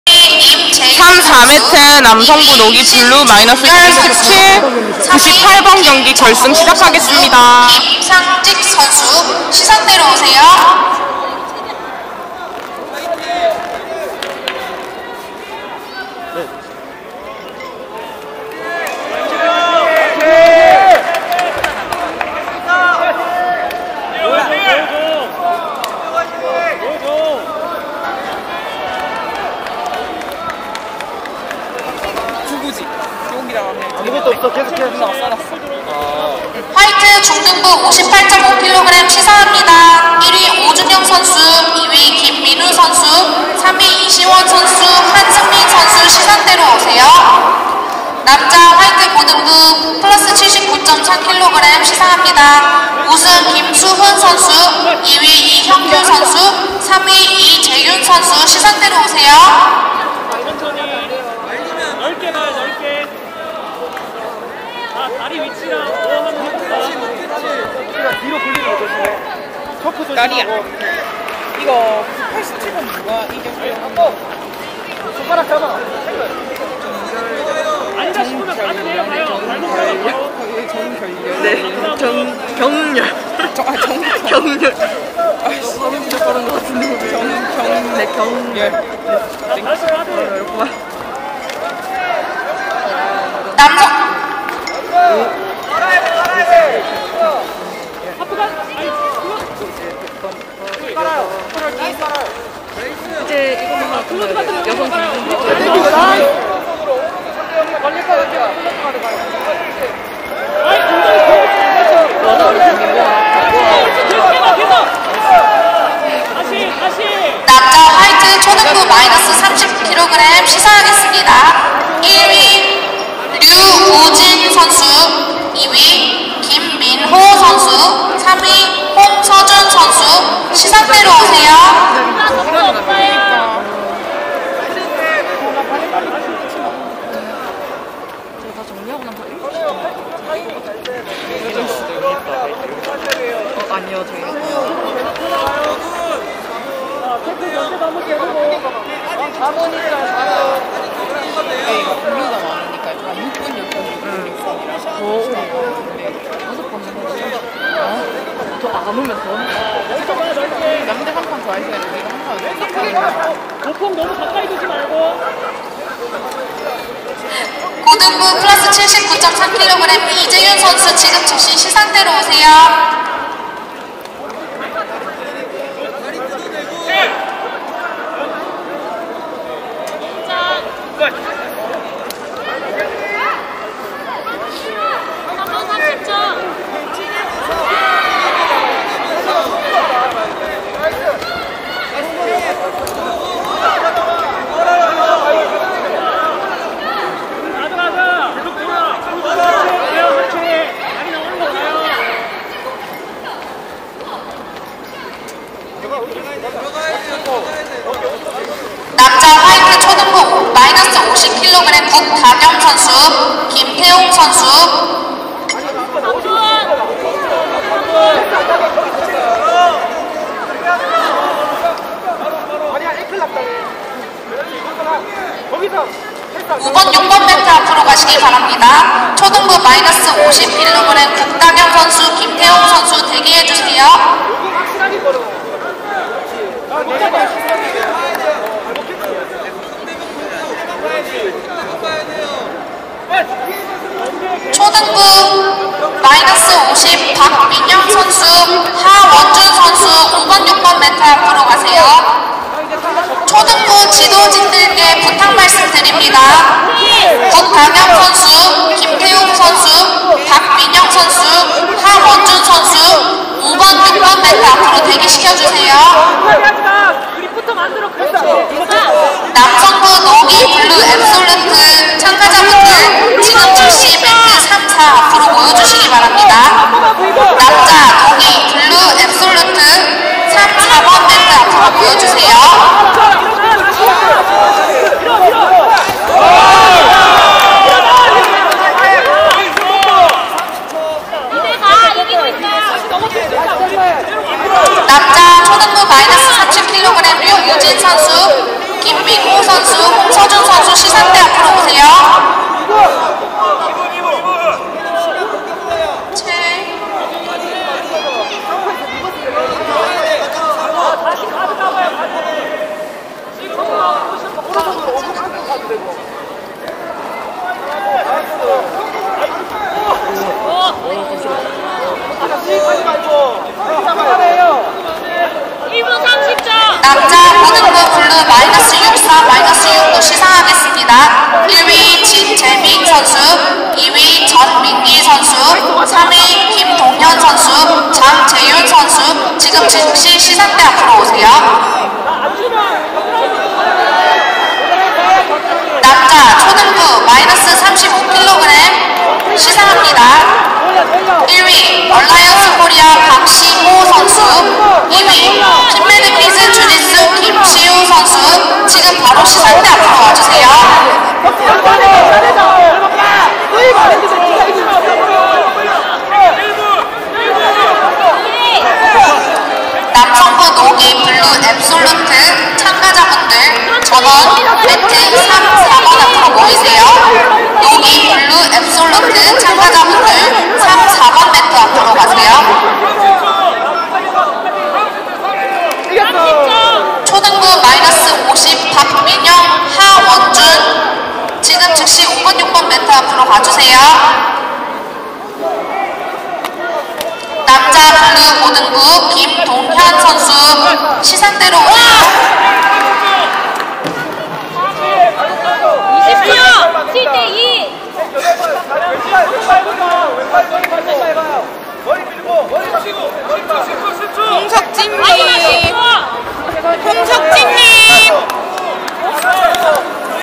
Mat 3-98 제갈동근 v 홍석진 남성부 노기 Blue -77kg 결승전 (로얄 마이너 부산 기장)
제갈동근(마르셀로가르시아 혼주짓수) v 홍석진(루카스레프리코리아 주짓수코어) 남성부 노기 Blue -77kg 결승전 경기입니다.